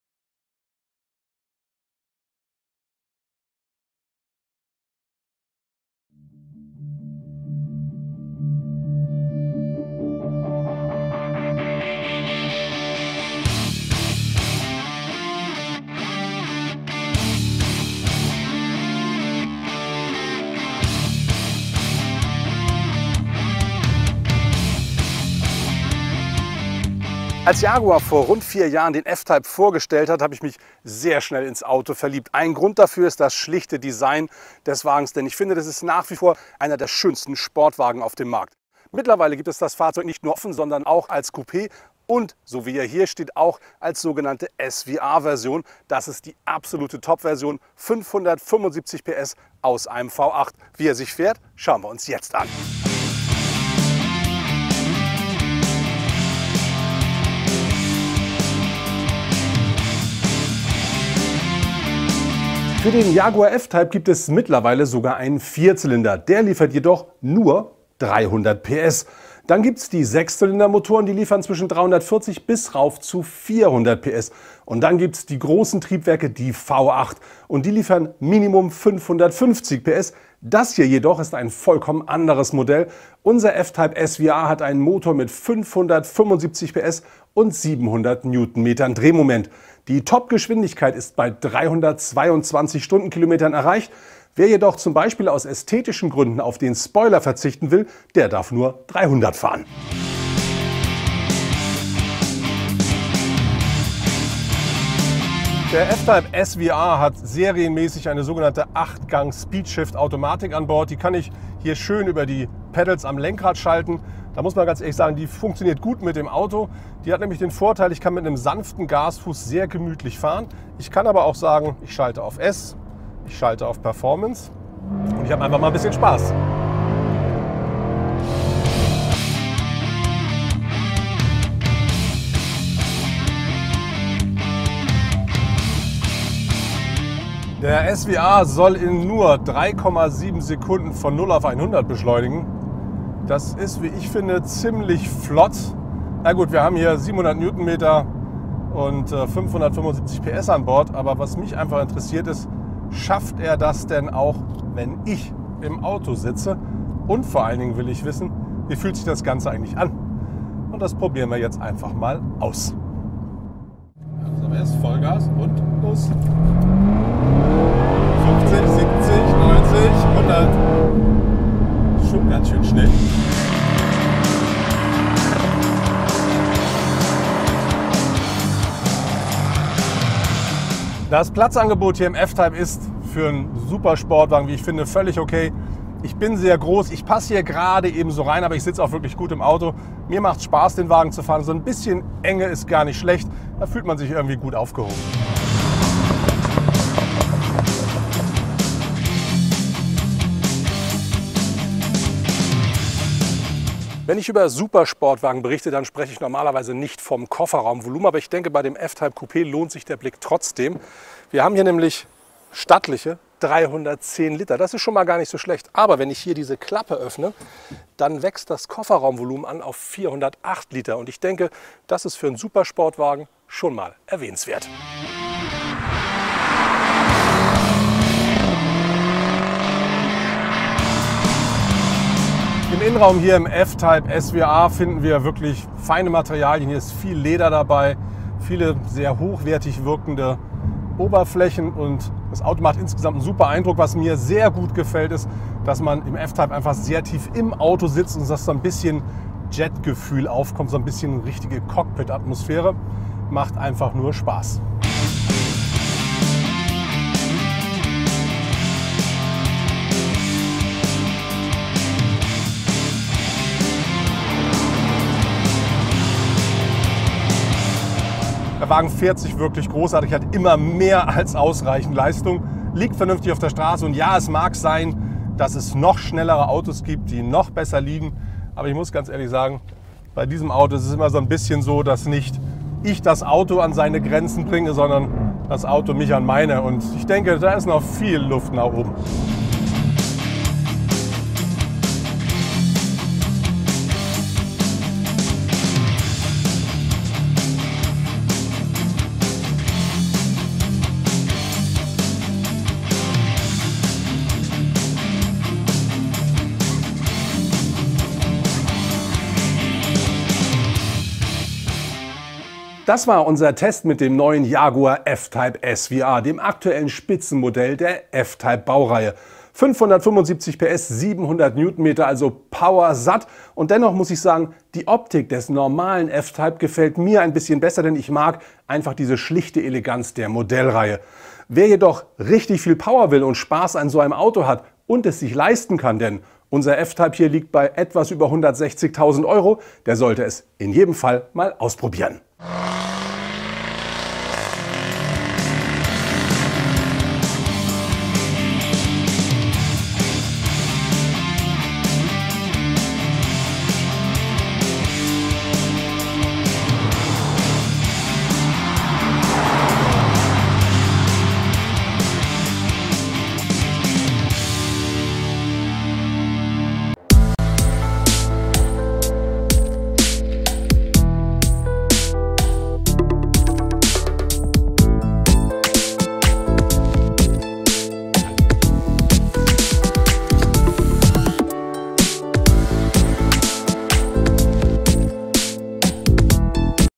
2018 Jaguar F-Type SVR Coupe |Testdrive & Review | Deutsch.
With the Jaguar F-Type Jaguar has made the way back to the real sports cars. And even the basic version of the two-seater, whether Coupe or convertible, always for a lot of attention. Their latest top version is the F-Type SVR, which not only visually stands out from the base model. With its 575 hp, 700 Nm and all-wheel drive, his performance is in a class of its own. What the sporty cat really offers and how the F-Type makes on the road shows my driving report.